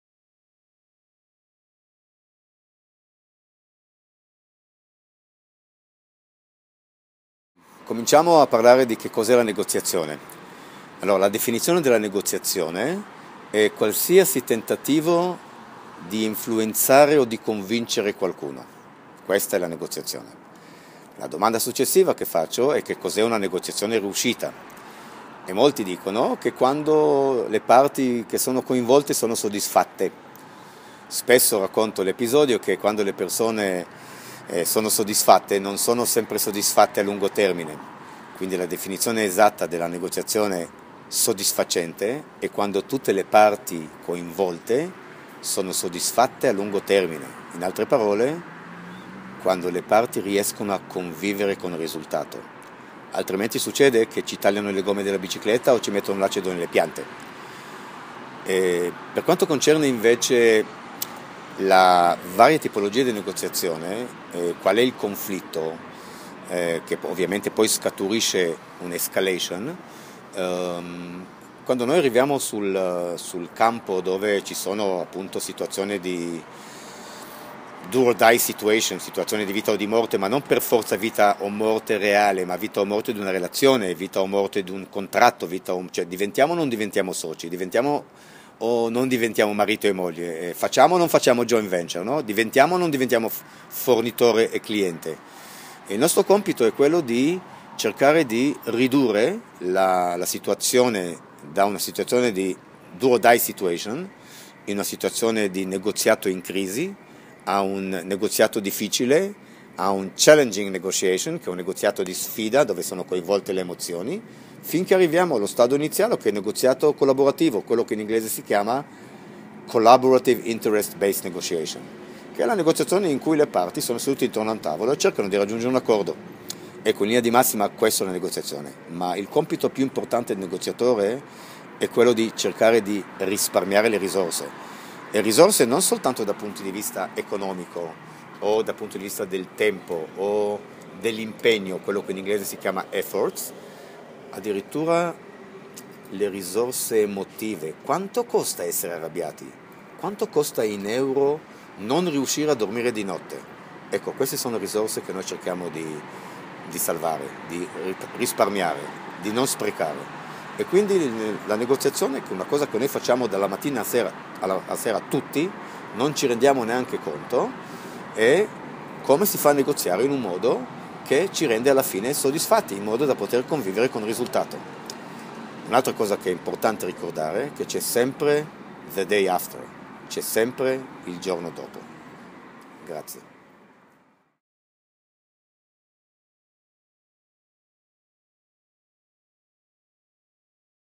ARIK STRULOVITZ
NEGOZIARE CON SUCCESSO PER INCREMENTARE LA REDDITIVITÀ E
LA FIDELIZZAZIONE DELLA CLIENTELA.
La negoziazione è un processo decisionale che si attiva quando per raggiungere i propri obiettivi è necessario trovare un accordo con uno o più partner con cui si hanno sia interessi comuni, sia contrapposti.
E’ fondamentale, quindi, per una buona riuscita della negoziazione, pervenire ad un risultato che sia soddisfacente per tutte le parti, evitando lo scaturire del conflitto e la conseguente interruzione delle relazioni.
Chiunque può negoziare, ma solo pochi lo fanno in modo professionale.